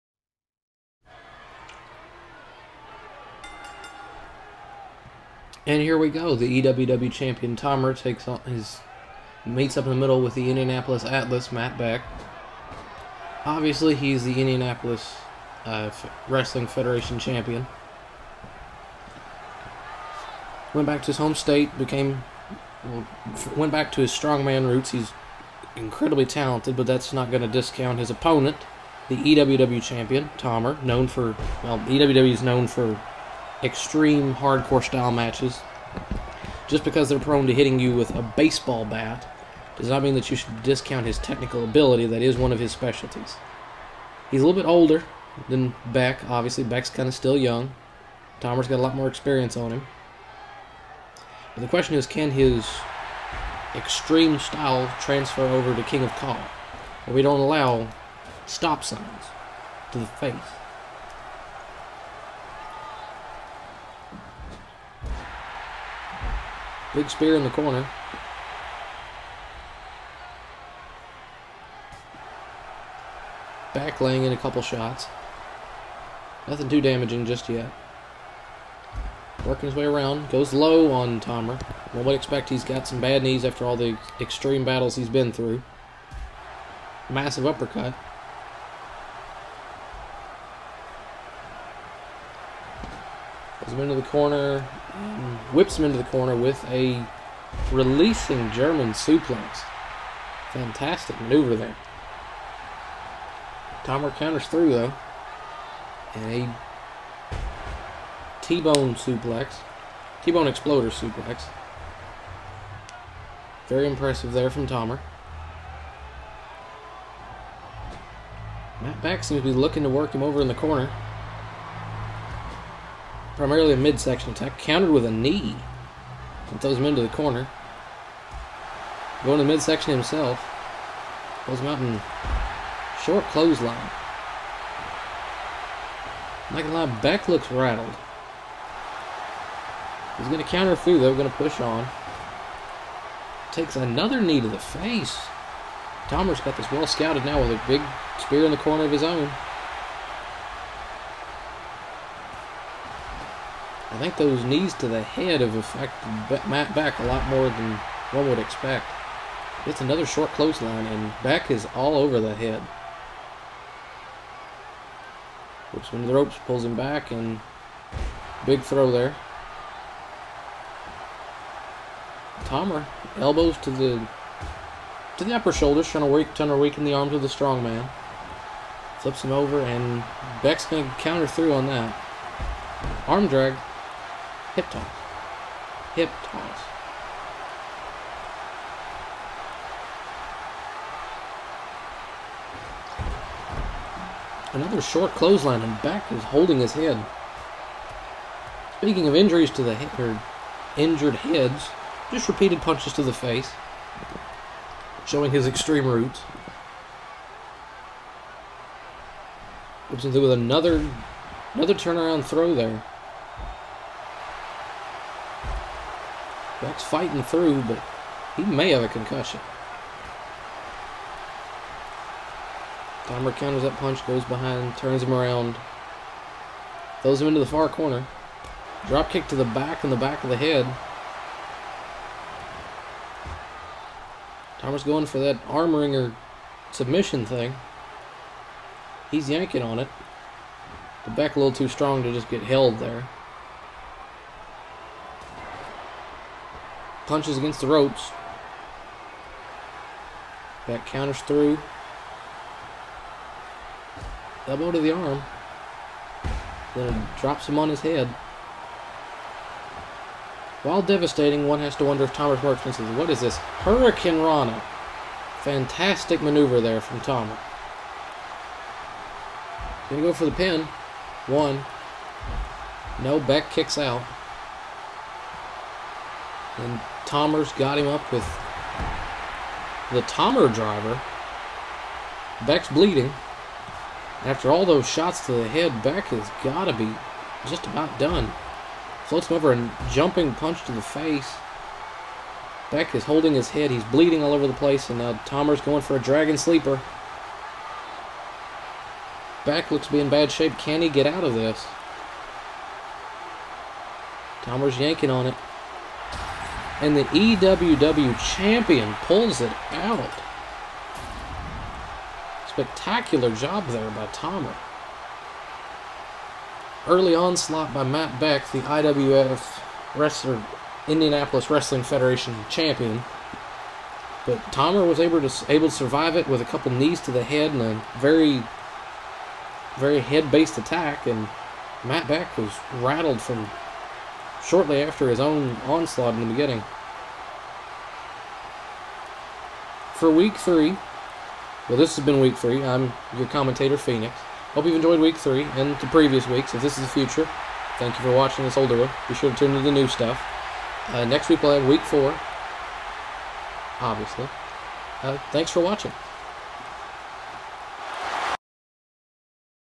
PTV - TOMER VS MATT BECK - King of CAW {HD}
Extreme Warfare Wrestling Owner and Champion, Tomer, battles reigning Indianapolis Wrestling Federation Champion, "The Indianapolis Atlas" Matt Beck.

Your daily gaming channel, PhoenixTV, bringing our subscribers new video game based content every day.